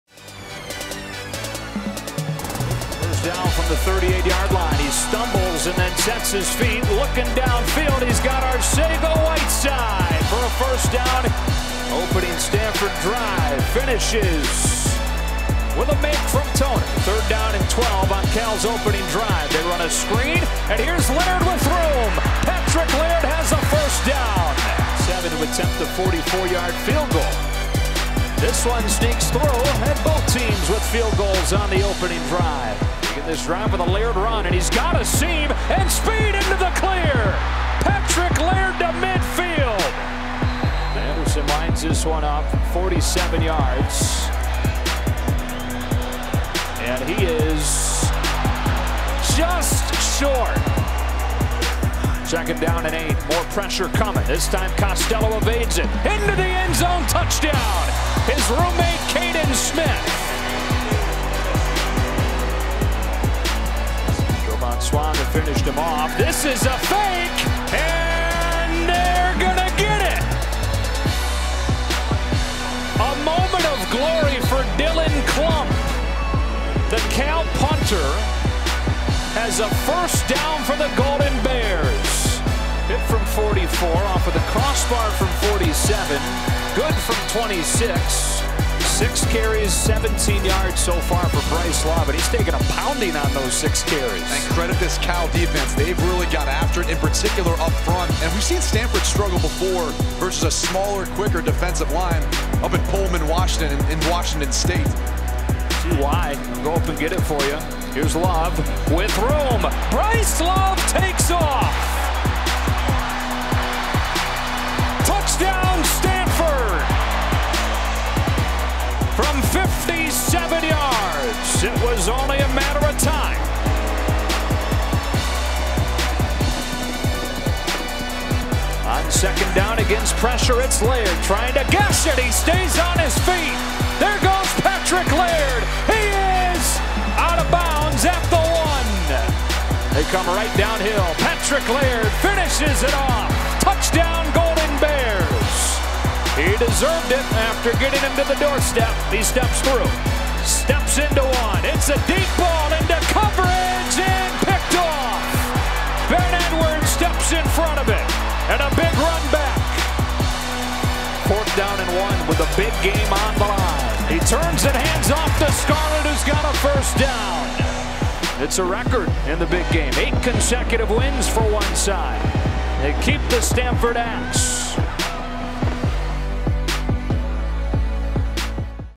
Highlights: Stanford football holds off California on The Farm for eighth consecutive Big Game...
Highlights from No. 22 Stanford's 17-14 win over California in the 120th edition of the Big Game on Saturday night. The win was the Cardinal's eighth consecutive victory in the rivalry. Stanford quarterback K.J. Costello completed 17 of 26 passes for 185 yards and a touchdown while running back Bryce Love tallied 101 yards and a touchdown.